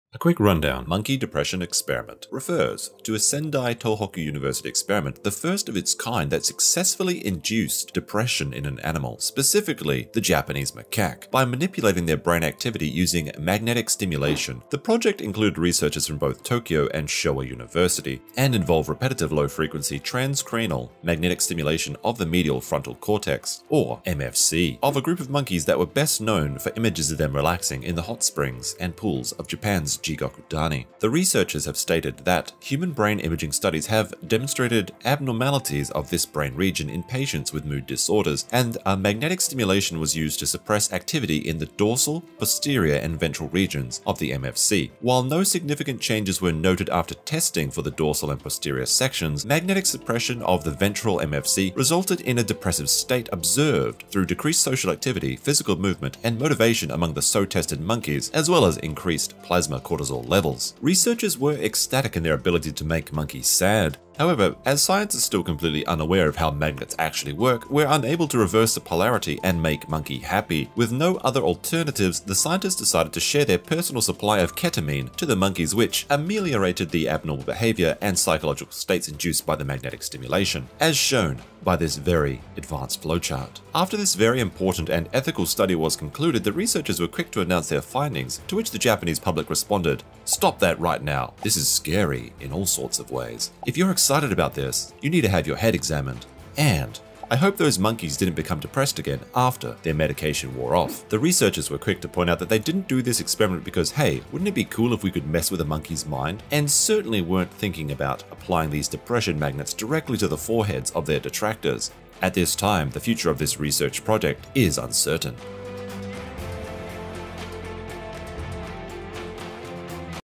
Monkey Depression Experiment | A Quick Rundown
A QRD on Sendai’s Tohoku University successfully inducing depression in monkeys by manipulating their brain activity using magnetic stimulation. The project, which researchers from the University of Tokyo and Showa University also contributed to, involved repetitive low-frequency transcranial magnetic stimulation of the medial frontal cortex (MFC) of Japanese macaque monkeys.
Audible Plus
Audible Premium
Kindle Unlimited
Amazon Prime
Amazon Prime Gifting